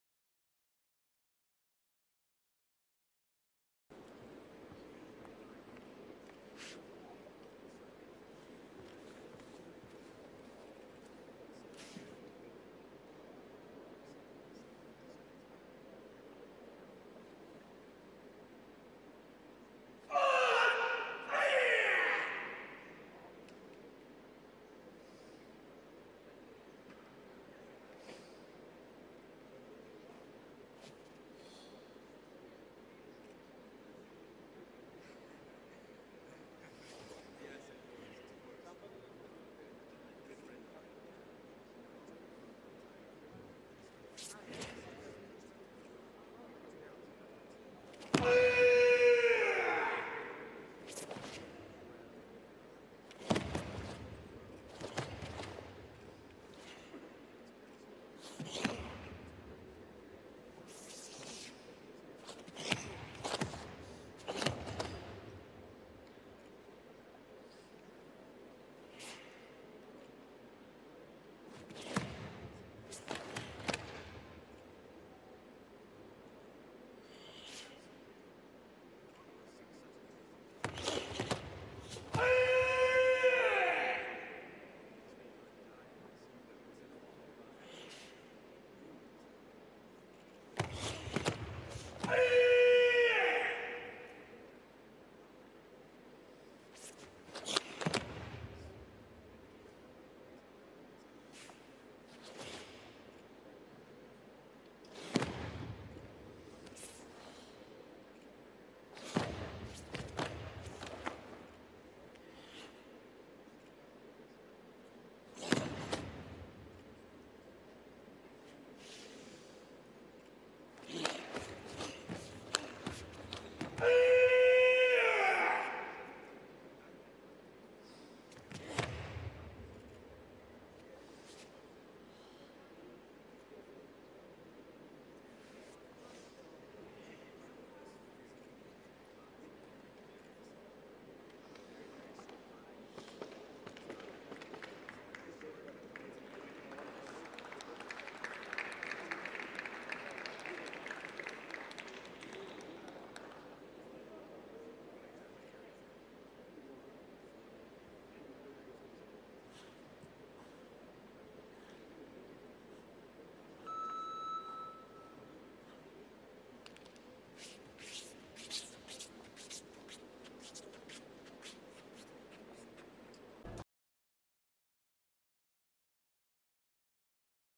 OHAN DAI - ARIEL TORRES GUTIERREZ ( USA) ,BRONZE MEDAL Bouts Match 1
KARATE1 PREMIER LEAGUE FUJAIRAH 2022